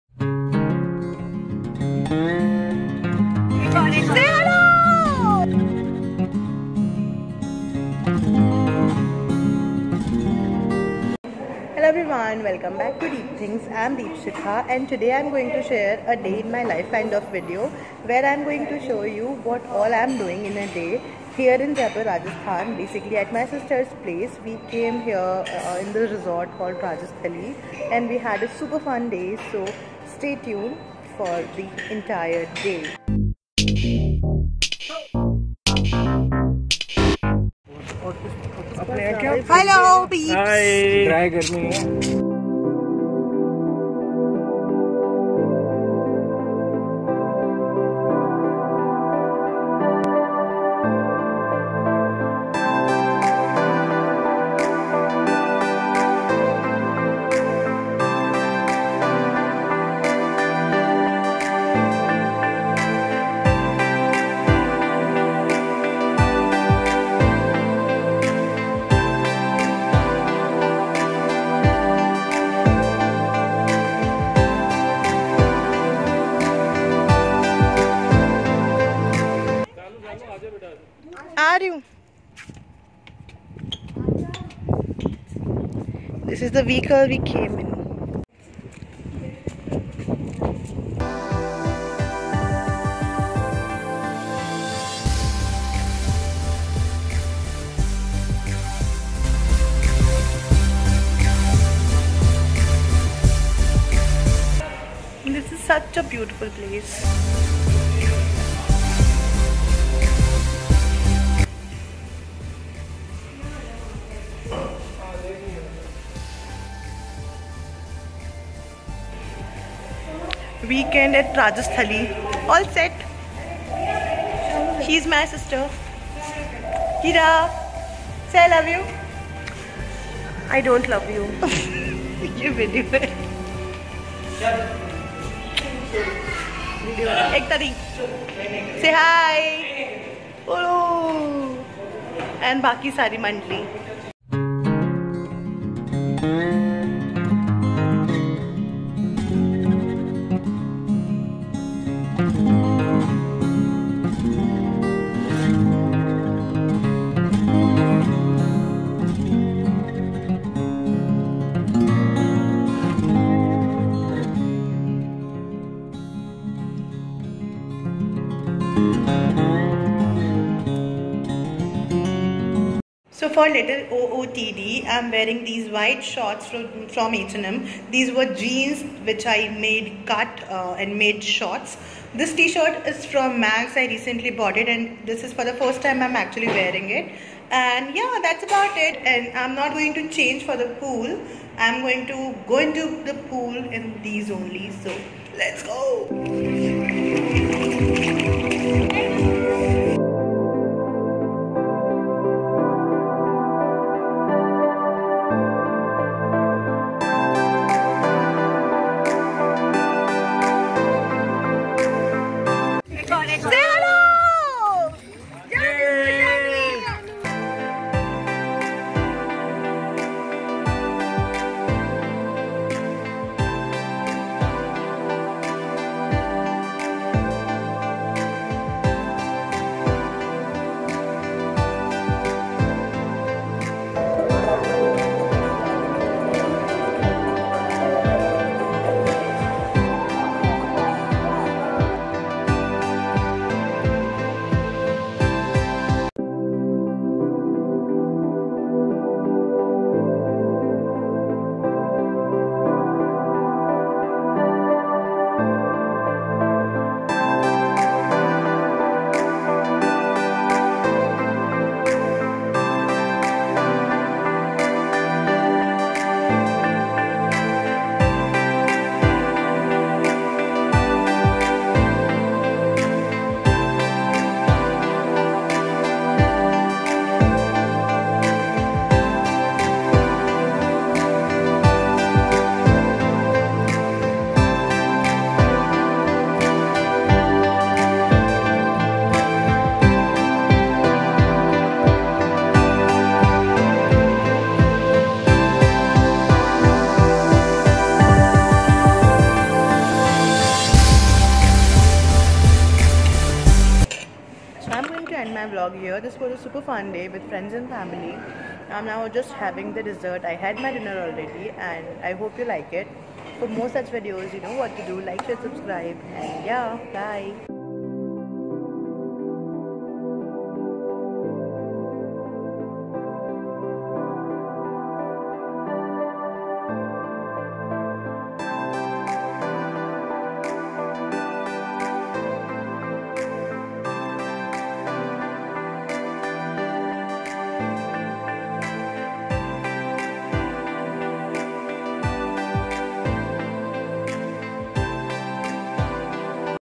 A day out at Rajasthali Resort || DeepThinks Vlogs
Hello everyone,
Sometimes out with family is such a stress buster in our lives. So I always take some time off once in a while and spend it with family.
I hope you'll like this vlog.
Do share such your moments in the comment section down below.

Deepshikha
XOXO XOXO